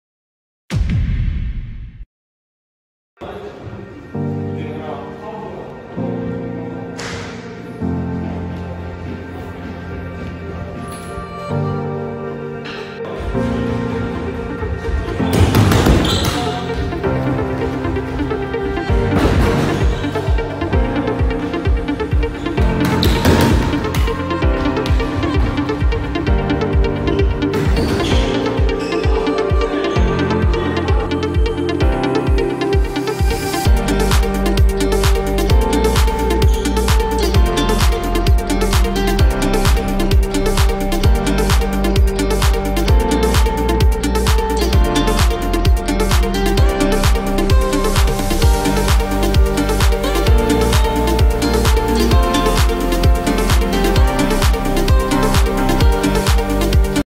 엉덩이왕 인서울가보자!!ㅣ5월모의테스트 10위 신장고 박준수 [스왓TV]
5월모의테스트 10위 신장고 박준수

20m왕복달리기 - 14.37초
배근력 - 236kg
싯업 - 72회
좌전굴 - 23.3cm

lovesanglee0